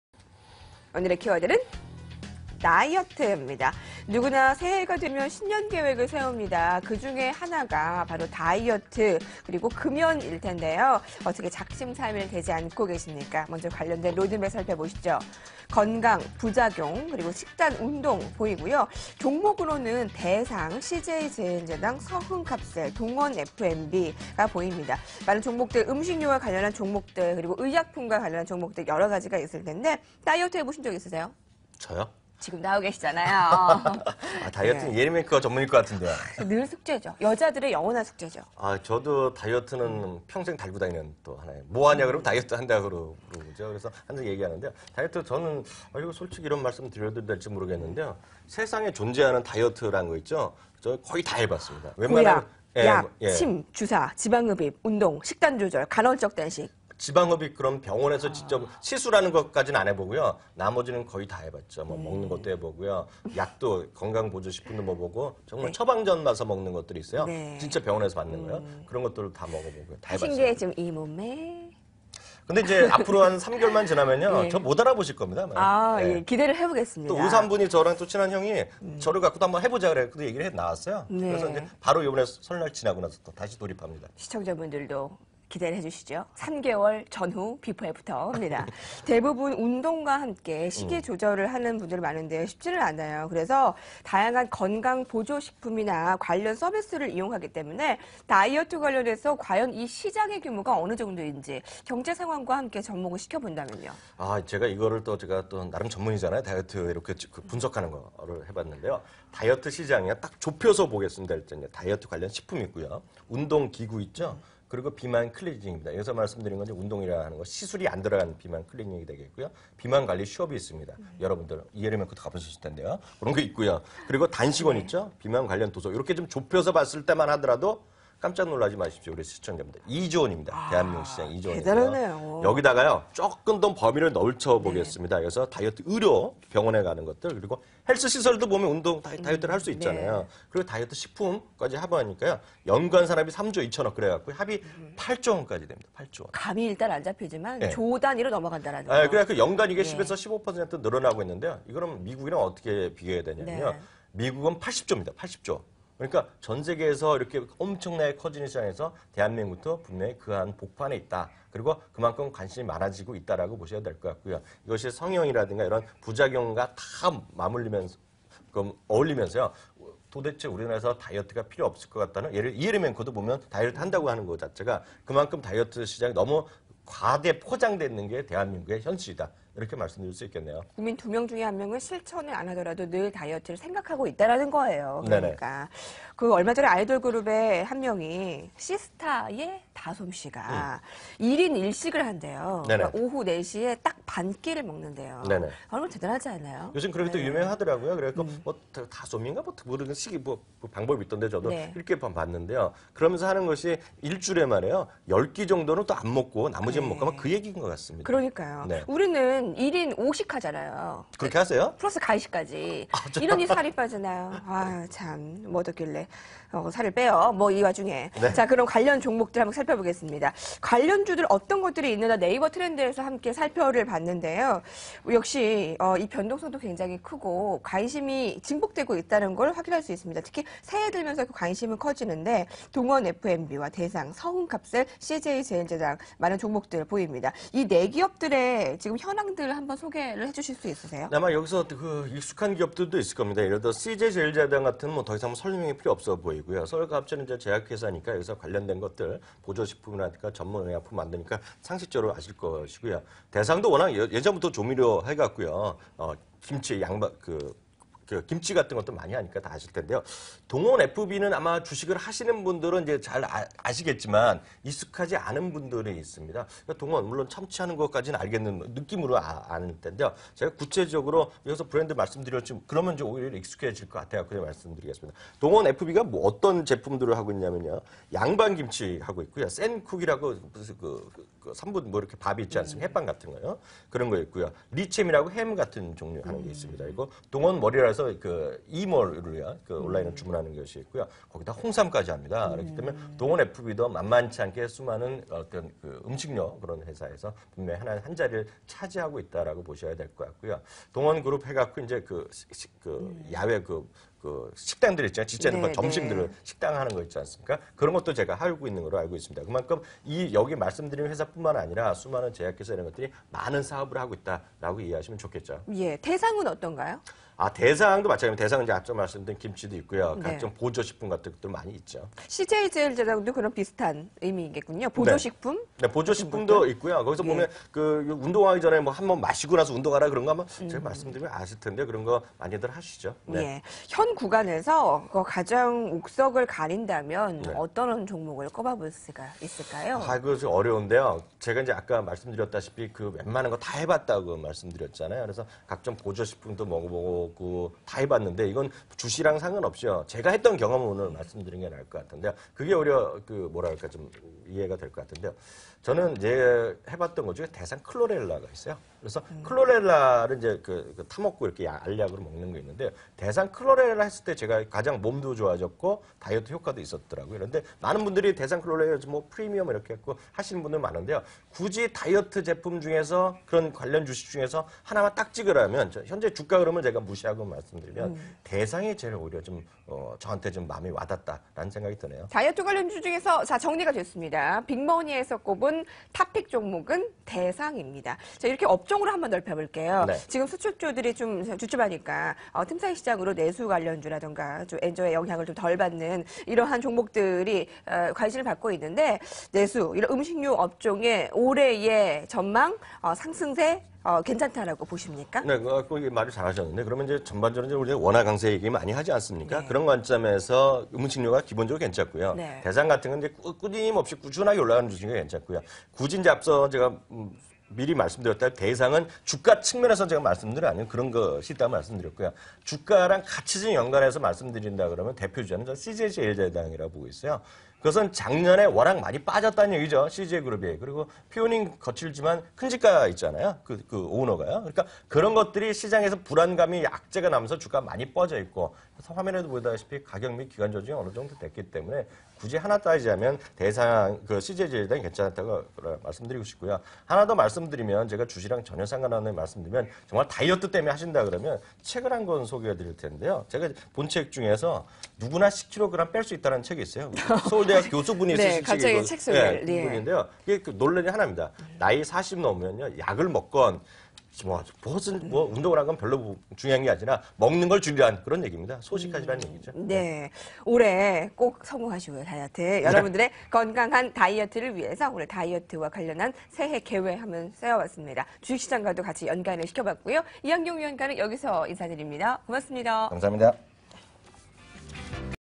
[빅머니]데이터 로드맵 '다이어트'
* 빅데이터 로드맵
최근 시장에서 이슈가 되고 있는 키워드에 대한 빅데이터 분석!
관련 키워드와 종목에 대한 전망과 투자전략을 제시한다.